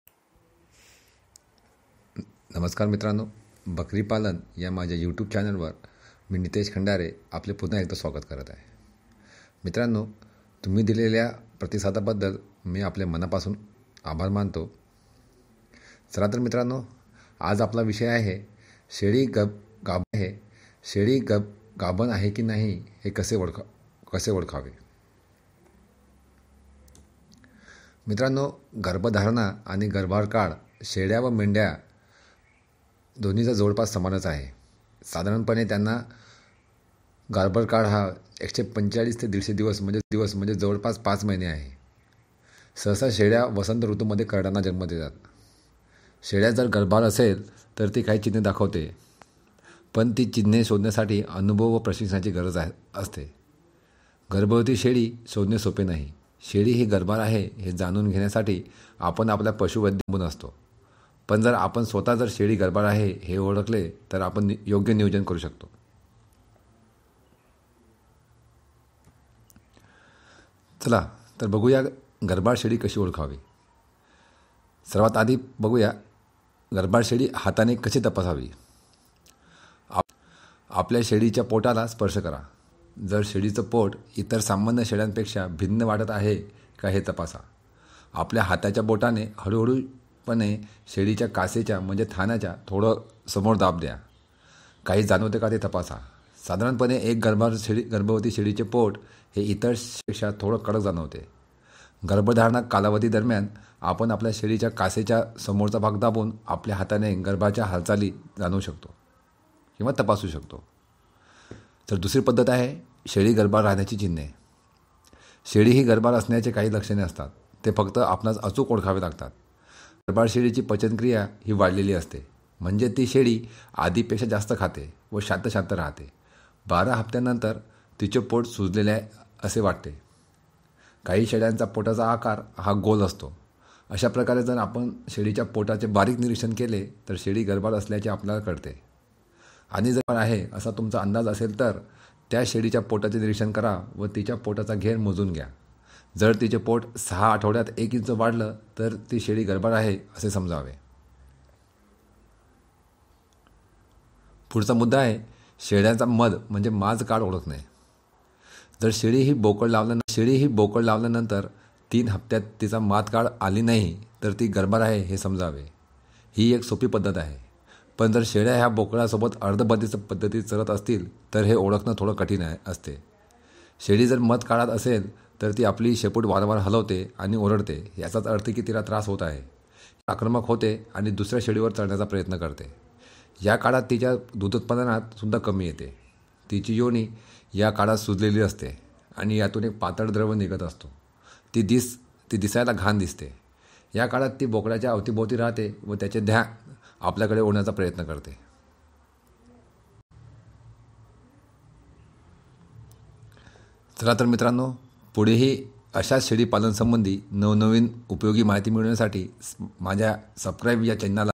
Goat Farming..शेळी गाभण आहे हे कसे ओळखावे।। गाभन बकरी कैसे पहचाने।।
शेळ्या जर गर्भार असेल तर ती काही चिन्हे दाखवते. पण ती चिन्हे शोधण्यासाठी अनुभव व् प्रशिक्षणाची गरज असते."गर्भवती शेळी शोधणे सोपे नाही आहे. शेळी ही गर्भार आहे हे जाणून घेण्यासाठी आपण आपल्या पशुवैद्यवर अवलंबून राहतो. पण जर आपण स्वतः जर शेळी गर्भार आहे हे ओड़खले तर आपण योग्य नियोजन करू शकतो.